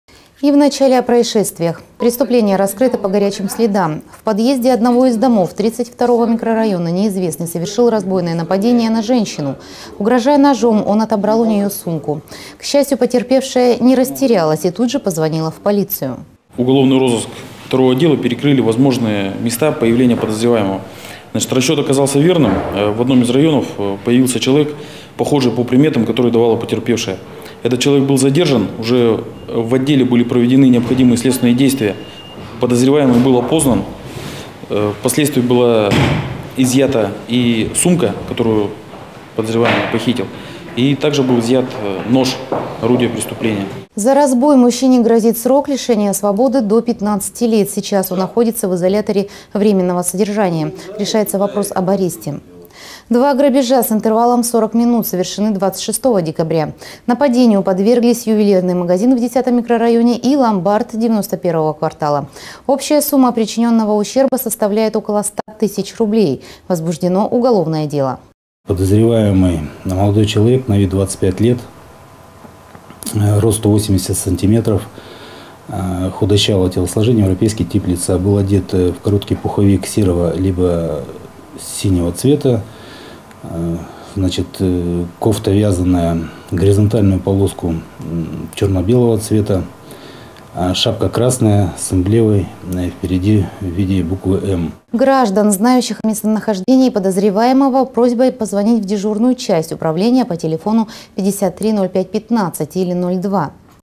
Задержан грабитель, вырвавший сумку у женщины
Преступление раскрыто по горячим следам. В подъезде одного из домов 32 микрорайона неизвестный совершил разбойное нападение на женщину.
Угрожая ножом, он отобрал у неё сумку. К счастью, потерпевшая не растерялась и тут же позвонила в полицию.
Алексей Окуловский, и.о. начальника отдела полиции №2:

- Уголовный розыск второго отдела перекрыл возможные места появления подозреваемого. Расчёт оказался верным. В одном из таких районов появился человек, похожий по приметам, которые давала потерпевшая, и он был задержан. Уже в отделе были проведены необходимые следственные действия. Подозреваемый был опознан. В последствии была изъята сумка, которую он похитил, а также нож - орудие преступления.
За разбой мужчине грозит срок лишения свободы до 15 лет. Сейчас он находится в изоляторе временного содержания, решается вопрос об аресте.